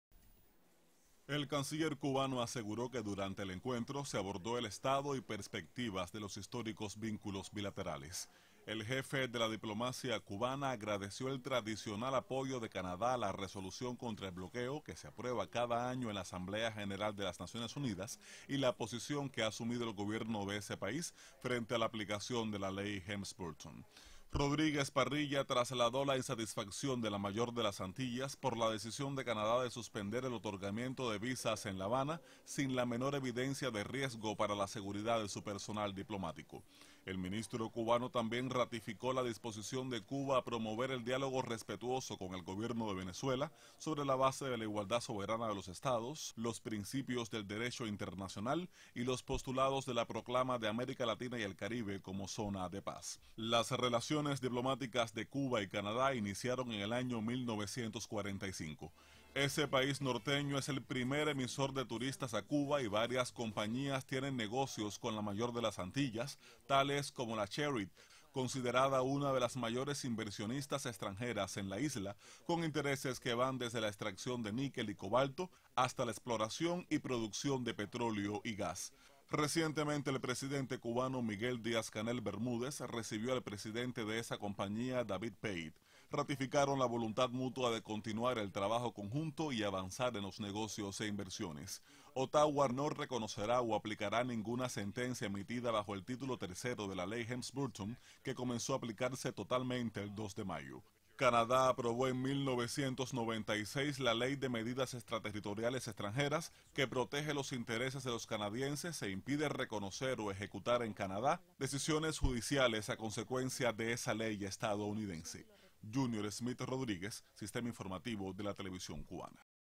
Recibió Bruno Rodríguez Parrilla a Canciller de Canadá
El ministro cubano de exteriores, Bruno Rodríguez Parrilla, recibió a su homóloga canadiense, Chrystia Freeland y a la delegación que le acompaña, quienes realizan una visita de trabajo a nuestro país.